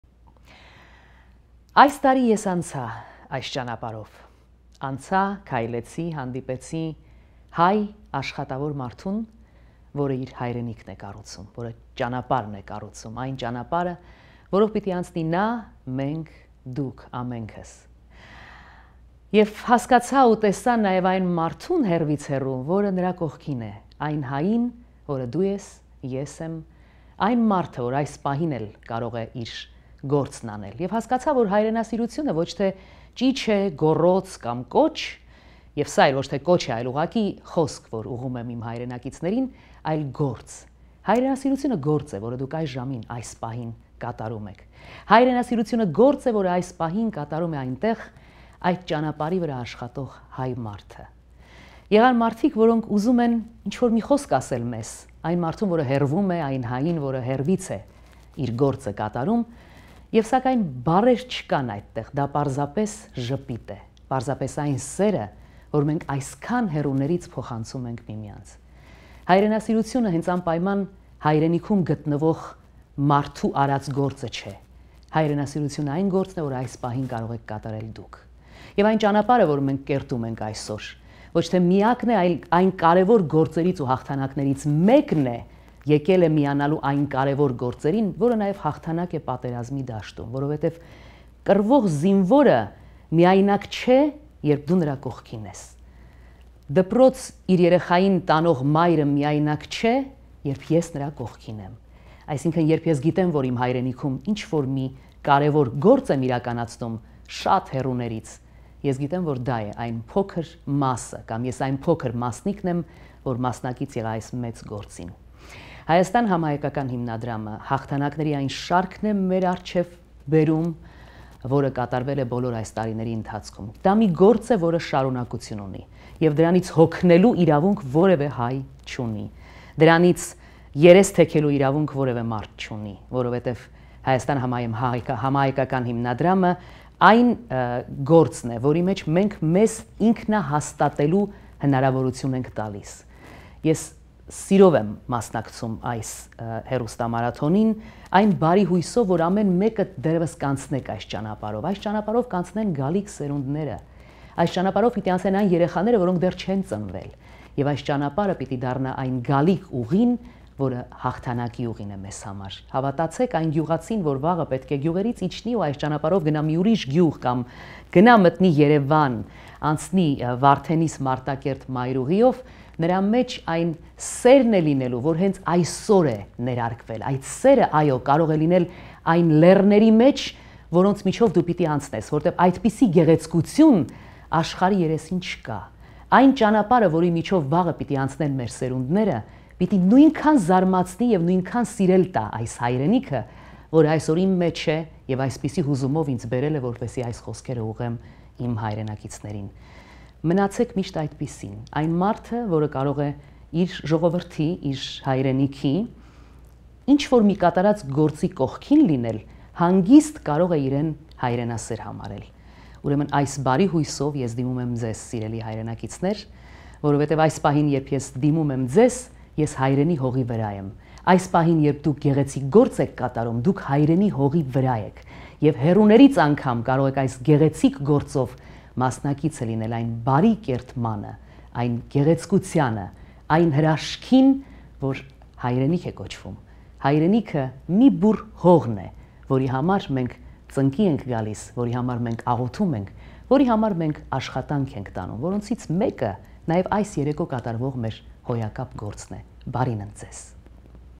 Nune Avetisyan On Vardenis-Martakert Highway
TV journalist Nune Avetisyan talks about her visit to the beautiful Vardenis-Martakert Highway, her interaction with the people living around and working on the highway, and the importance of donating to Armenia Fund.